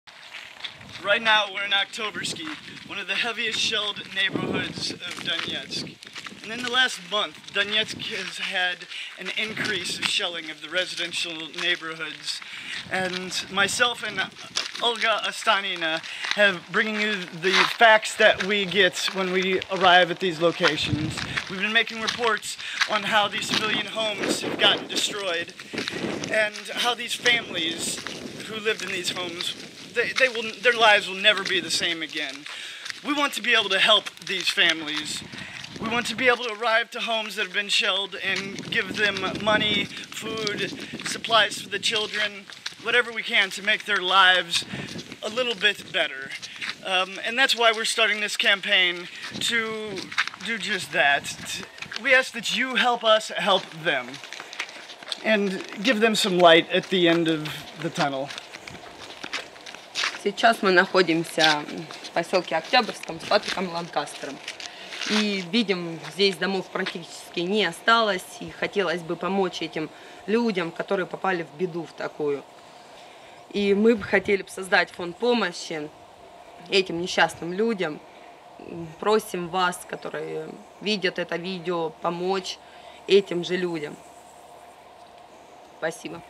Help recent shelling victims in Donetsk by donating to this crowdfunding campaign
Help Patrick Lancaster and I help recent shelling victims in Donetsk on indiegogo by donating